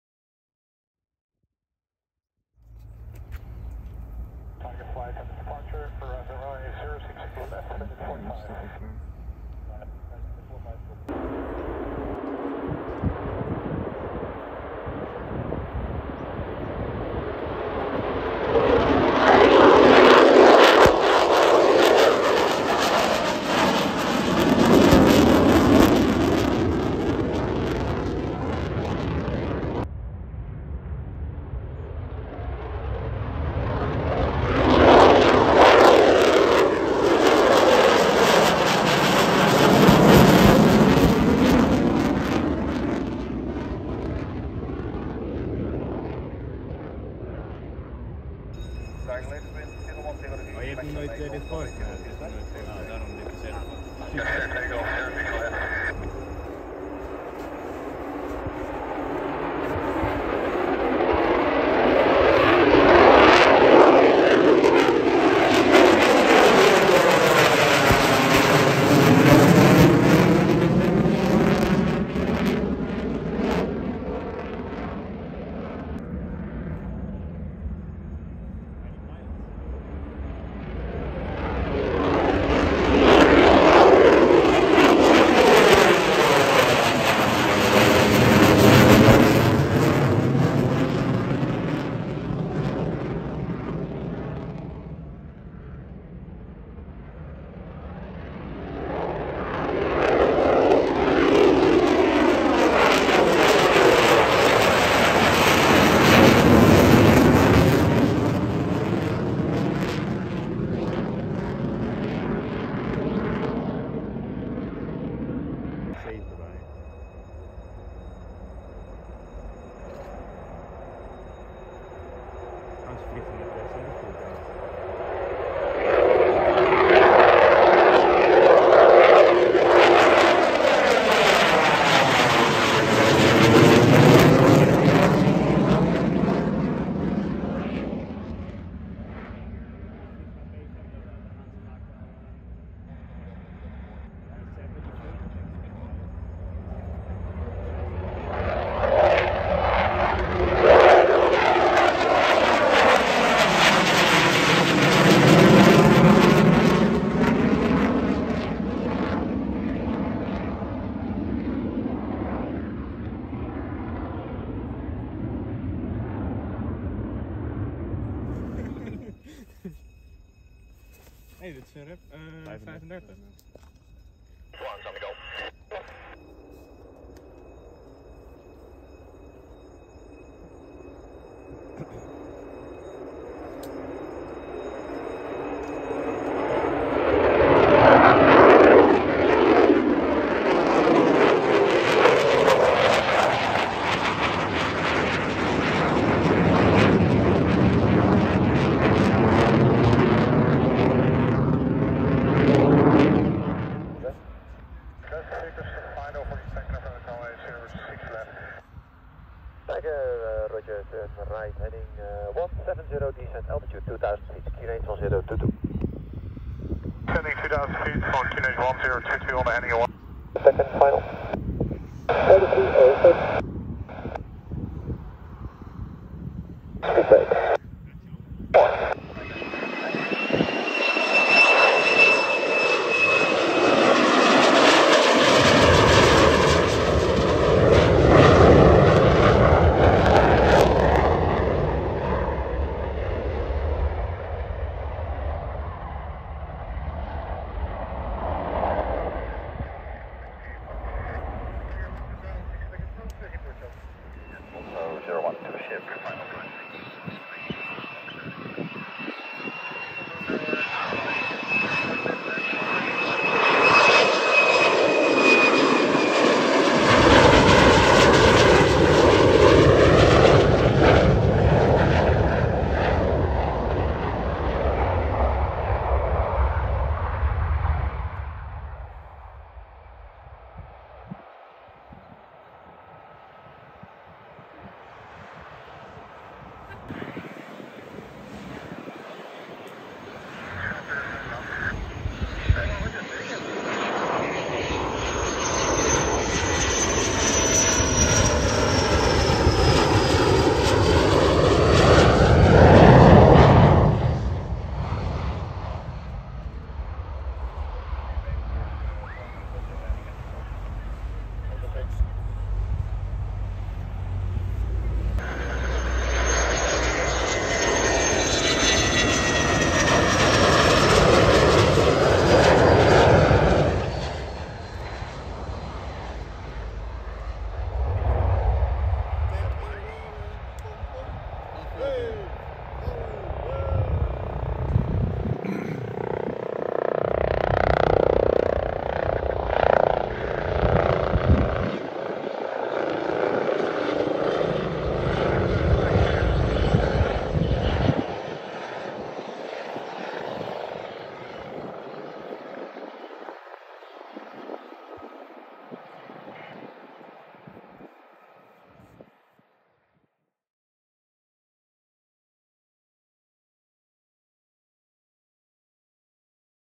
Volkel Airbase August 2024
Visit to Volkel Airbase in August 2024, See F16's en F35's takeoff and landings. Filmed on iPhone 13 Pro.